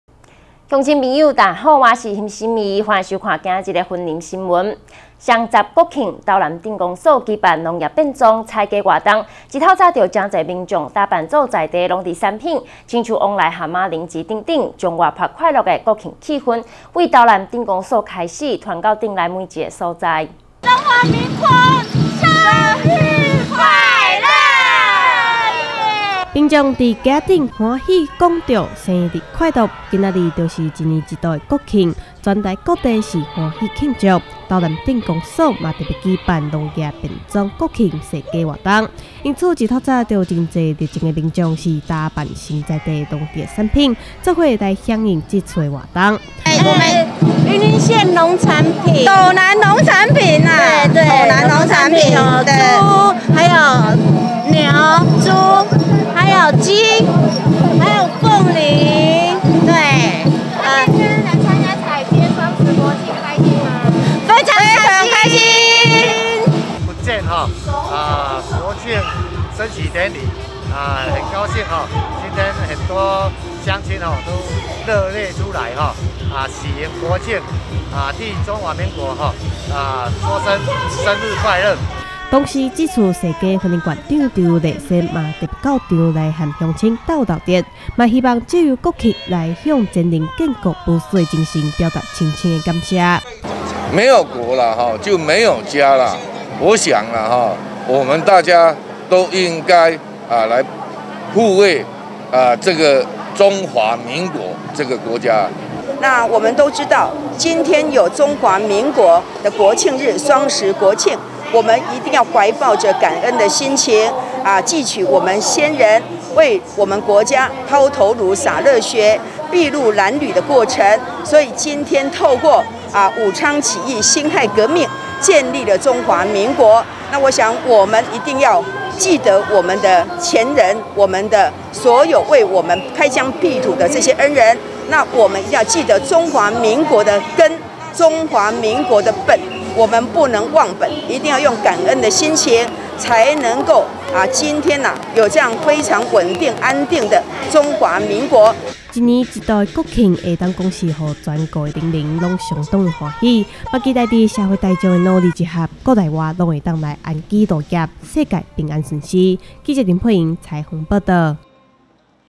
歡慶國慶民眾變裝農產超熱情！雲林縣長張麗善感念前人建國精神 －雲林新聞網
今日就是雙十國慶，為此，斗南鎮公所舉辦農業變裝踩街活動，一大早就有很多民眾是打扮成在地農特產品，親像旺來和馬鈴薯等等，將活潑愉快的國慶氛圍從斗南鎮公所開始傳遞到鎮上的每個角落。